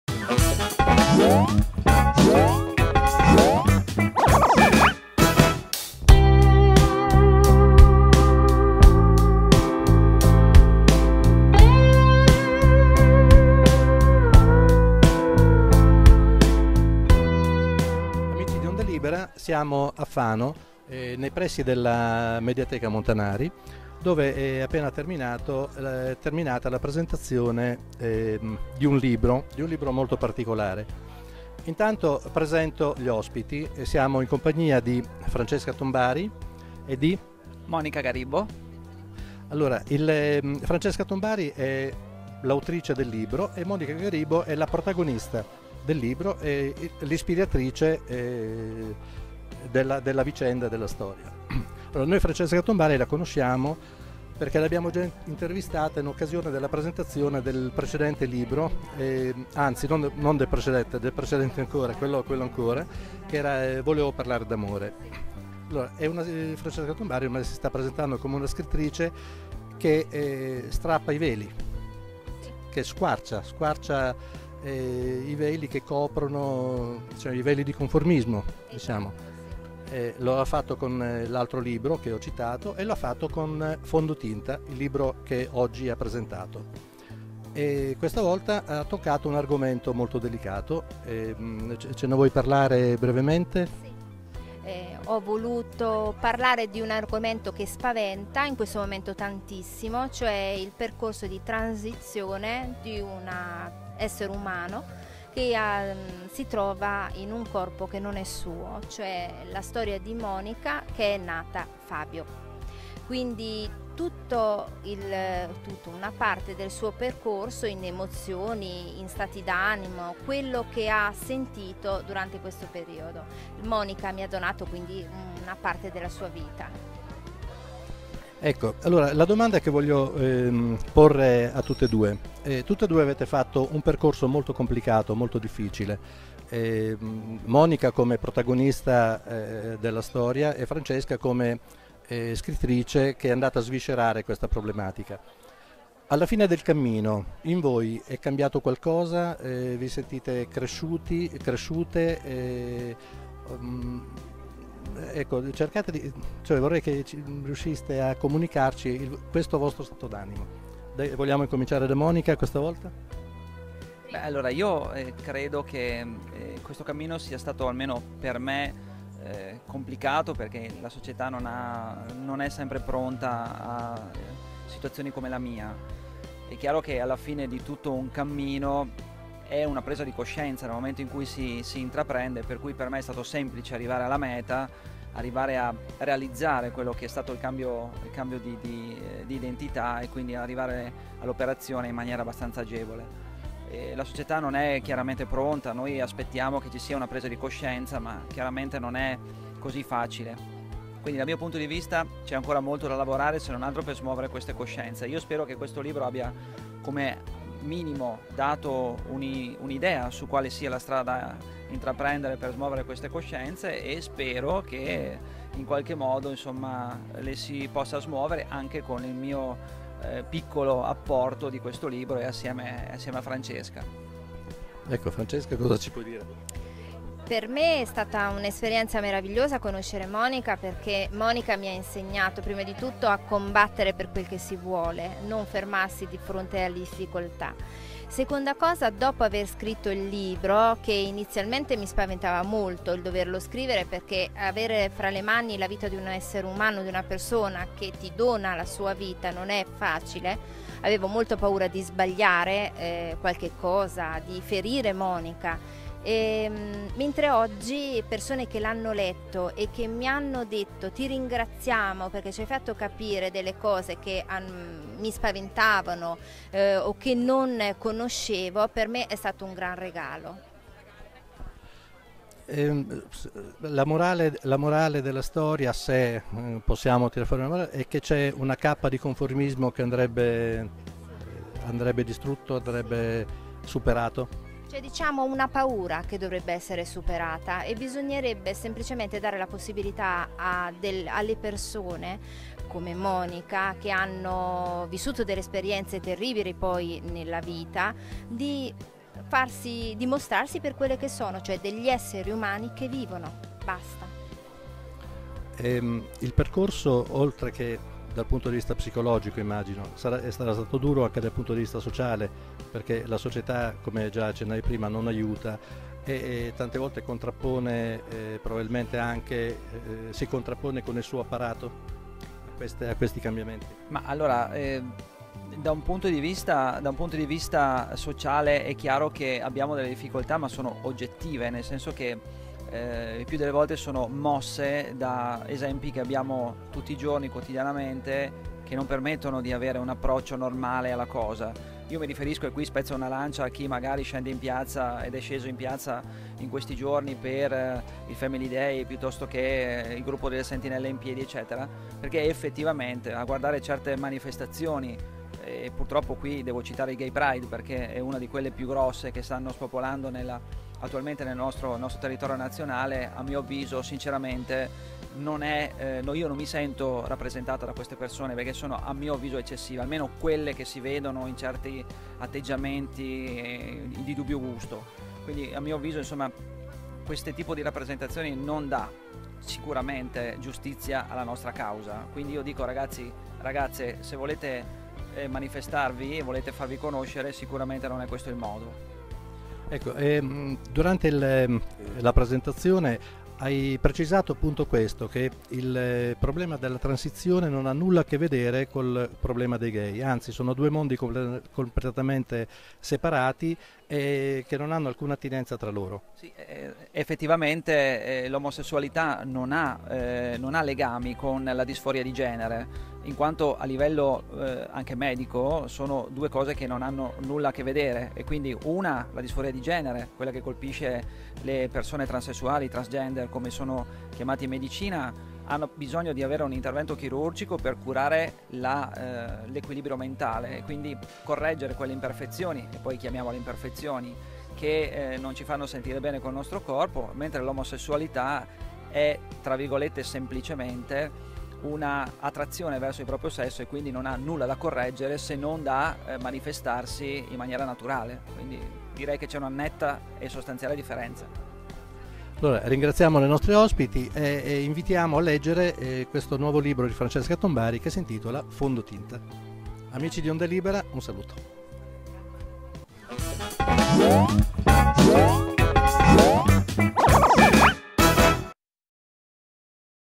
Fano - MeMo - Presentazione del libro "Fondotinta" di Francesca Tombari
Il 9 luglio 2015 Francesca Tombari ha presentato il suo nuovo libro, Fondotinta (Damster Edizioni) col quale, attraversando il mondo della "disforia di genere", ci ha raccontato la storia, vera, di Monica Garibbo  (Fabio alla nascita), facendoci rivivere il difficilissimo percorso di TRANSizione della protagonista.
All'incontro, condotto da Silvia Bonci e dalla piccola Mia, sono intervenute Francesca Tombari e Monica Garibbo le quali hanno dialogato a lungo con il numeroso pubblico presente.
Il problema del transessualismo ha impegnato per anni la dottrina e la giurisprudenza italiana, fin dalla promulgazione della legge 14 aprile 1982, n. 164, disciplinante la rettificazione dell'attribuzione di sesso. Con la sentenza della Corte Suprema di Cassazione del 20 luglio 2015, la problematica sembra aver imboccato la giusta via per una definitiva soluzione giuridica. Con tale sentenza, infatti, è stato elaborato il seguente principio:  “[i]l transessualismo si richiama ad un paradigma complesso in base al quale l’interazione di fattori biologici, psicologici e sociali influenza la costruzione dell’identità di genere. La chirurgia in tale prospettiva non è la soluzione ma solo un eventuale ausilio per il benessere della persona.”, salvaguardando in tal modo diritti fondamentali della persona e dando piena applicazione all'art. 32 della nostra Carta Costituzionale.
Contestualmente è giunta la sentenza della Corte europea dei Diritti umani con la quale l'Italia è stata sanzionata per non aver legiferato in materia di unioni civili tra persone dello stesso sesso.
La soluzione giuridica per il pieno riconoscimento dei diritti delle persone LGBT è quindi ormai alle porte.
Per la soluzione delle problematiche personali invece il cammino non può che essere individuale e, come ci dice Monica, passa necessariamente per la consapevolezza in quanto: "è sempre la coscienza che risolve ogni problema".